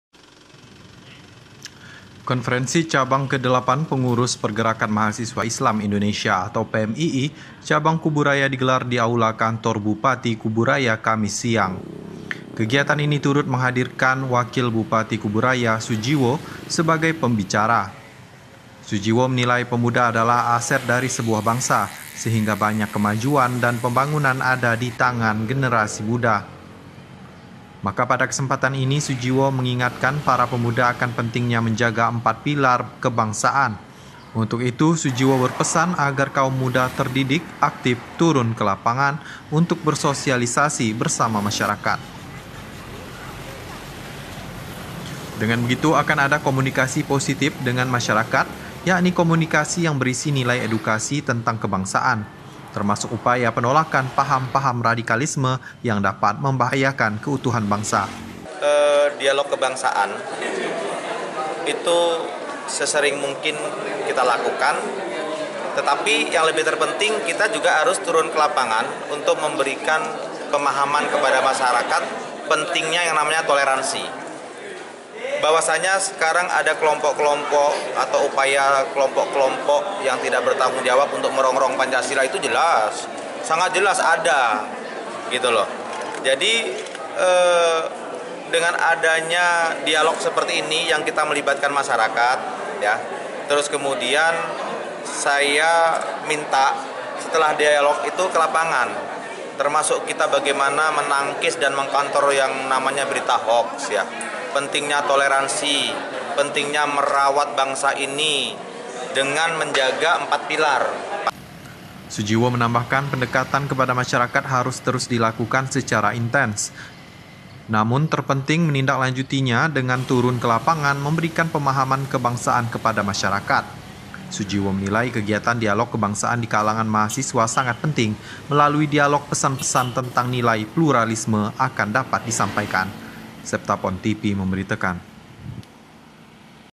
Sujiwo Ajak Pemuda Lawan Radikalisme
Wakil Bupati Sujiwo ajak para pemuda melawan paham radikalisme yang dapat menghancurkan empat pilar kebangsaan. Hal ini disampaikan saat menjadi pembicara di acara kofrensi cabang ke-8 Pengurus Pergerakan Mahasiswa Islam Indonesia atau PMII Cabang Kubu Raya